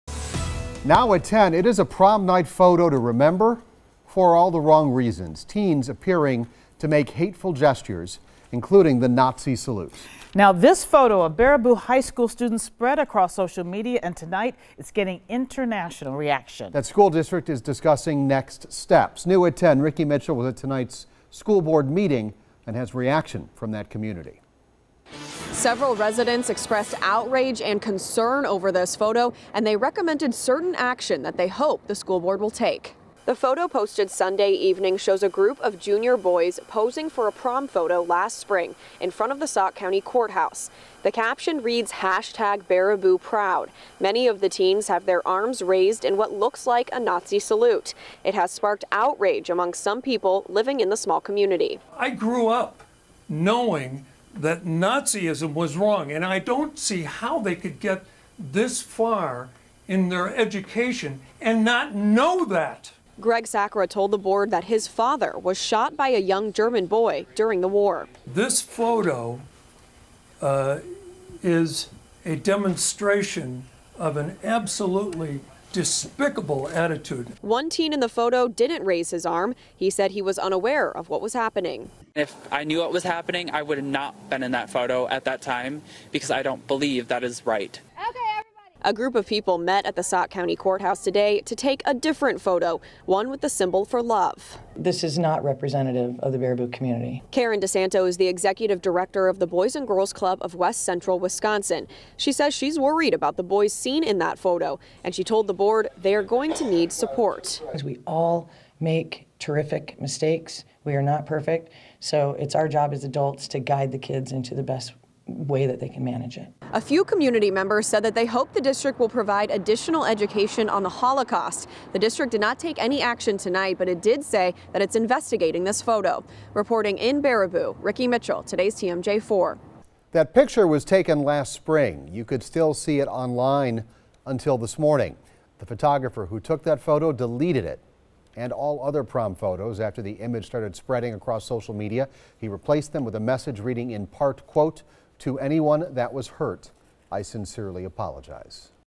Public demands action from Baraboo School Board for Nazi salute photo
The Baraboo School District is discussing next steps after a photo went viral showing junior students, with many appearing to be giving the Nazi salute.